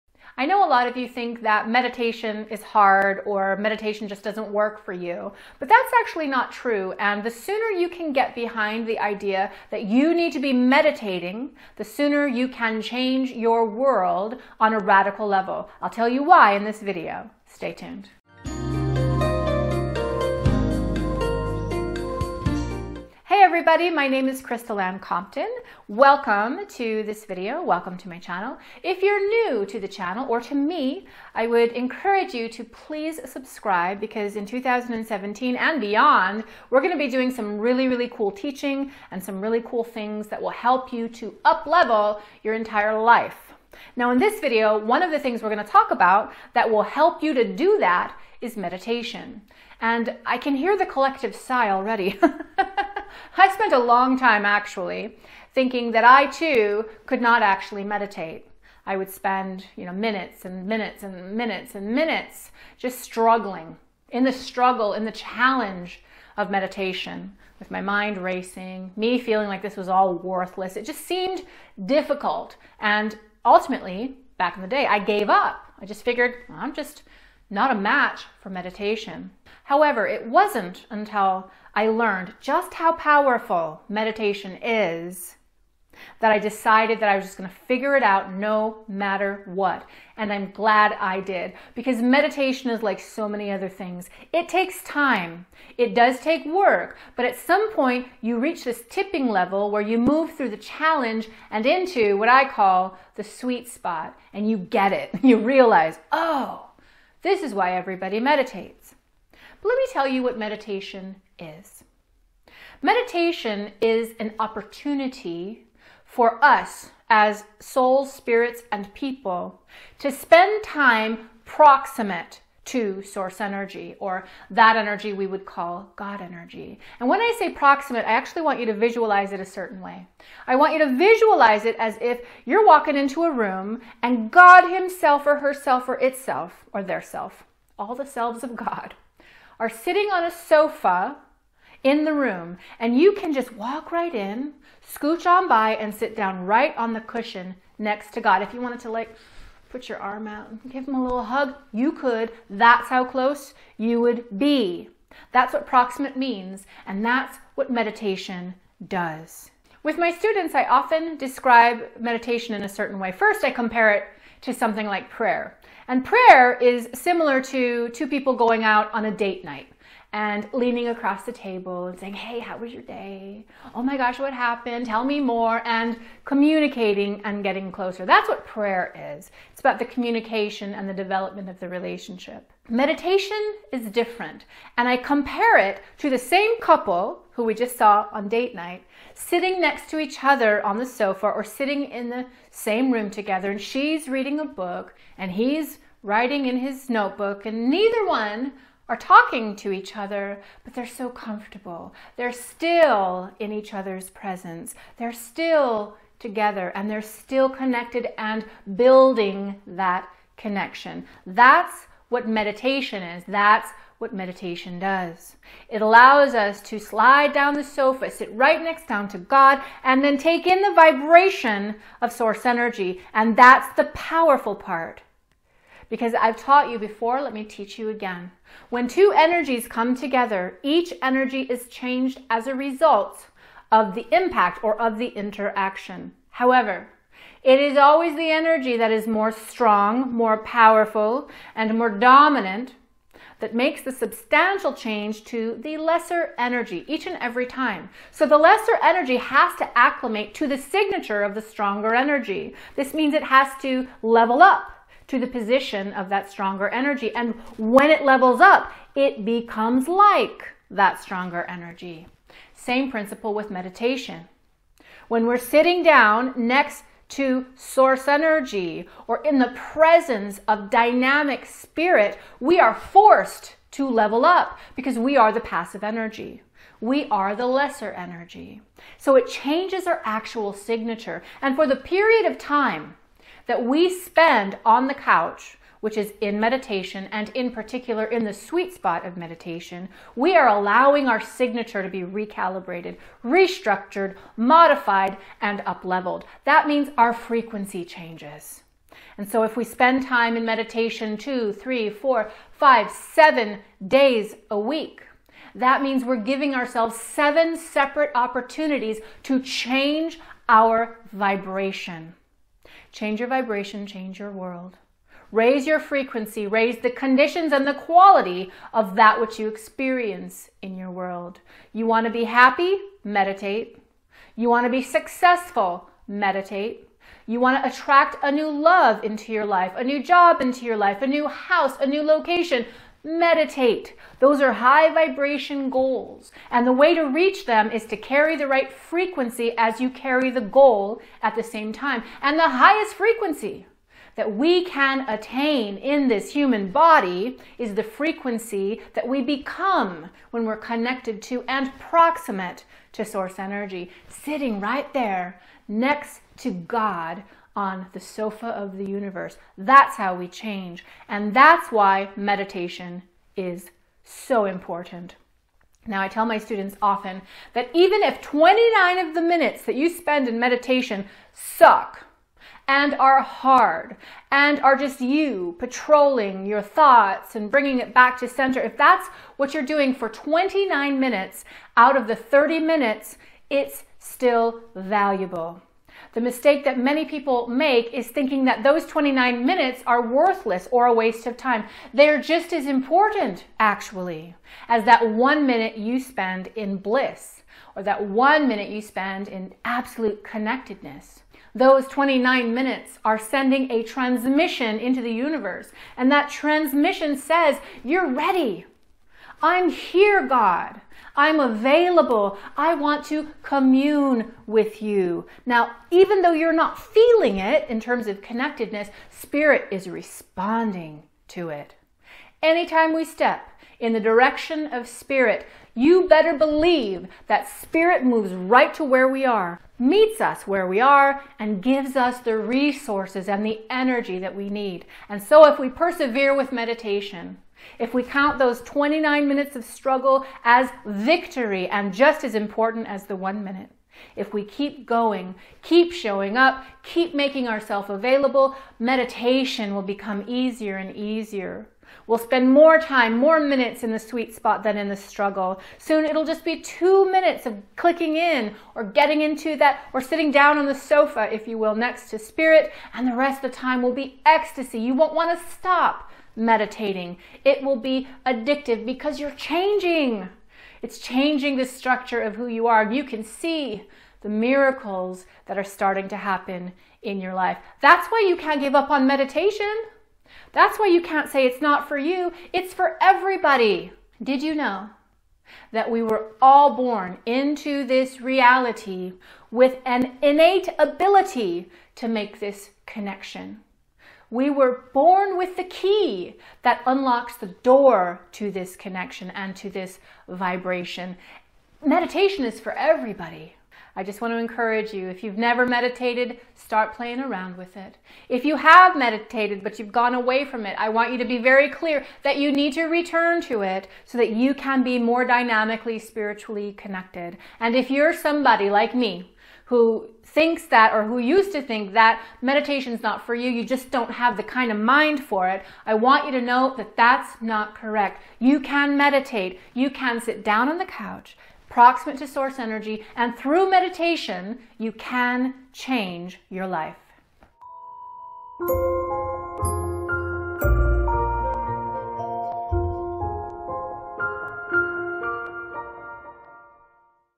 Why You MUST Meditate -- Even If You Think You Can't
Why you should meditate: I can't tell you how many times I've heard people say that they can't meditate. If you feel the same, let me be the first to tell you that it's not true! In fact, it's the BELIEF that you can't meditate that is keeping you from a DYNAMIC and EVIDENTIAL relationship with Source Energy. We can ALL meditate -- and we should!